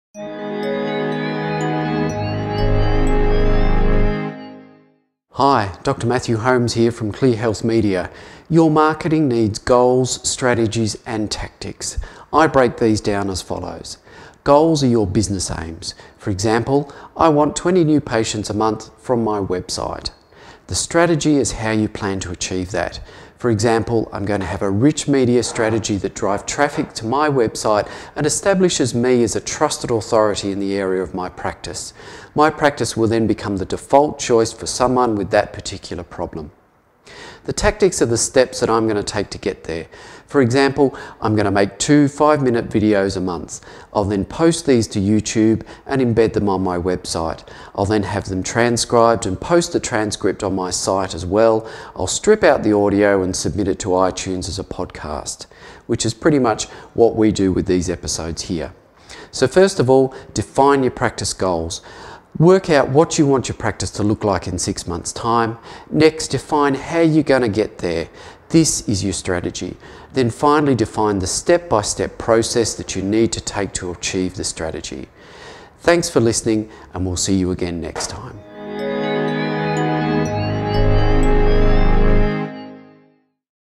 Goals, Strategy and Tactics in Marketing Your Practice
A marketing plan is essential to get the most from your promotion.  In this short video Dr Matthew Holmes quickly reviews the difference between goals, strategy and tactics and how they apply in your marketing.